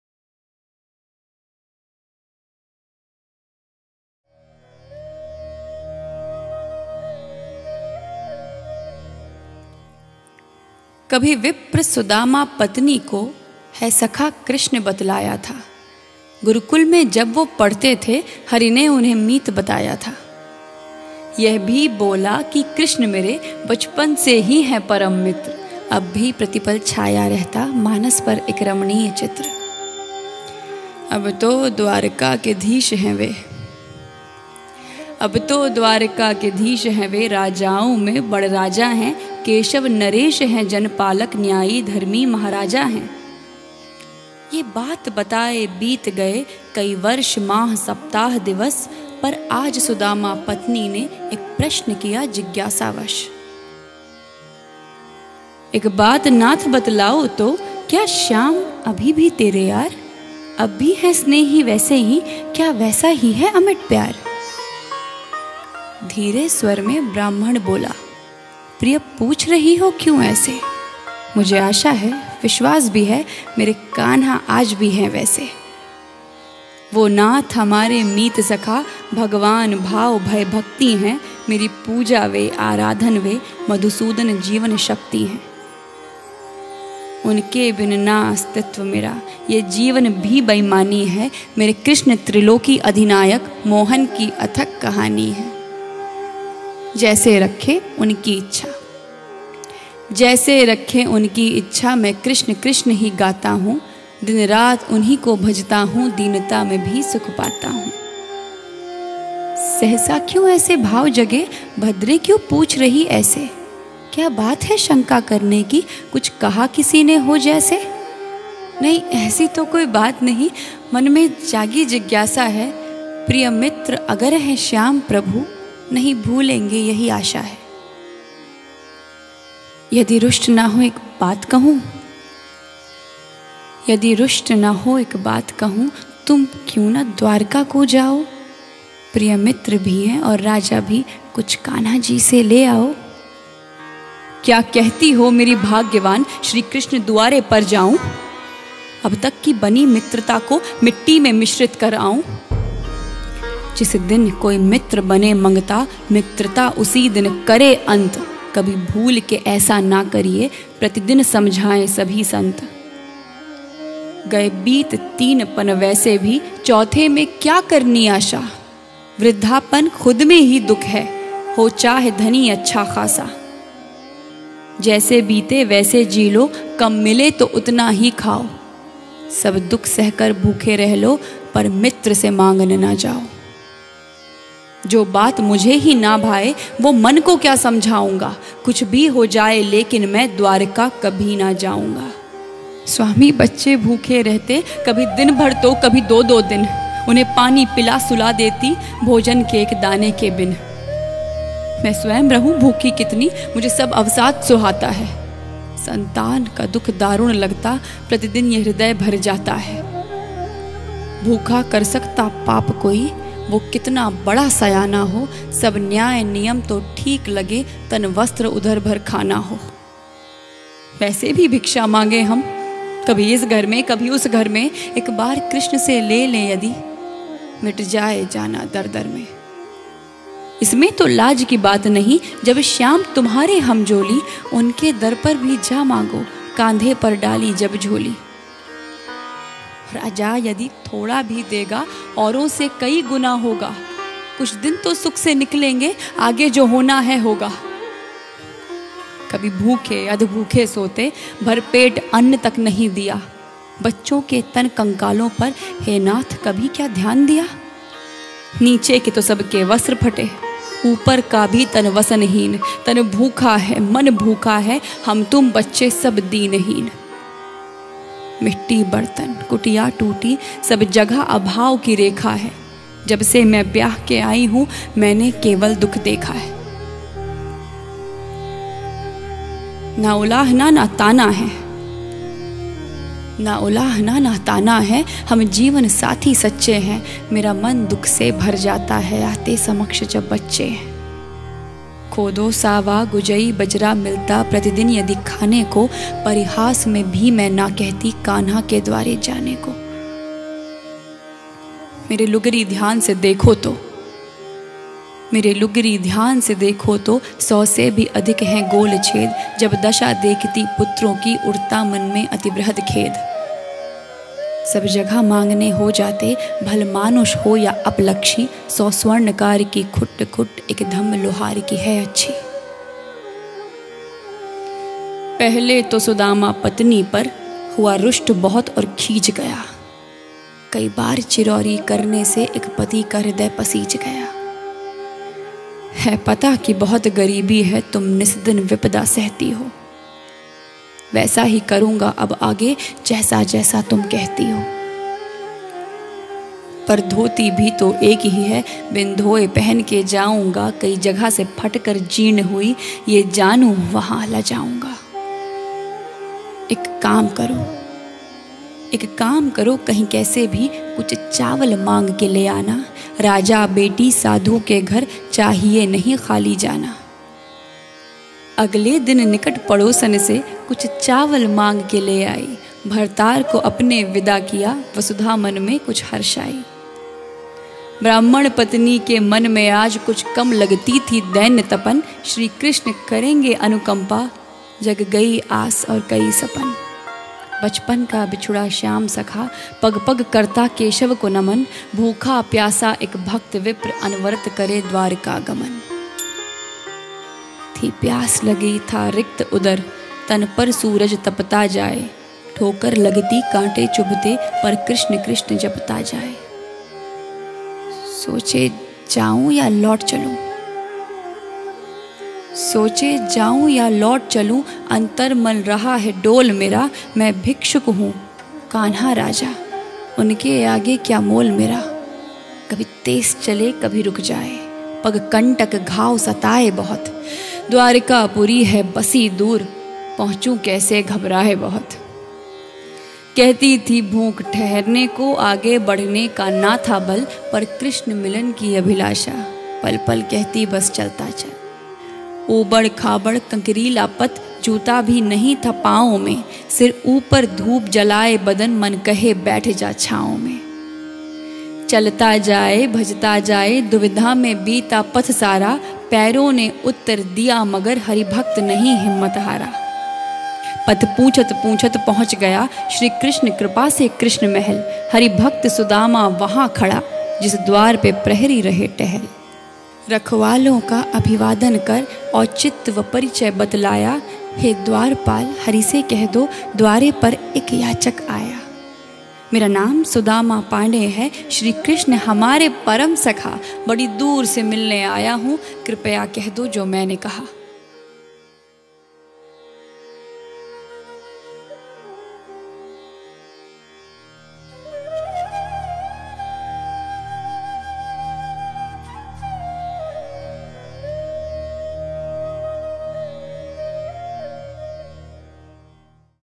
Kahan Sudama Bapuro... | कहाँ सुदामा बापुरो | Satish Srijan | Vaishnavi Sharma | New Krishna Poem
part 1| Kahan Sudama Bapuro... | भाग १ | कहाँ सुदामा बापुरो | Satish Shrijan | Vaishnavi Sharma | New Krishna Poem

Poet : Satish Srijan
Recitation By : Vaishnavi Sharma
Special Thanks to @LNiteshKumar
Video & Music Production : 9Frequencies Studios

2021. All copyrights are reserved with 9Frequencies Records Pvt Ltd.
©️®️ Raag Vaishnavi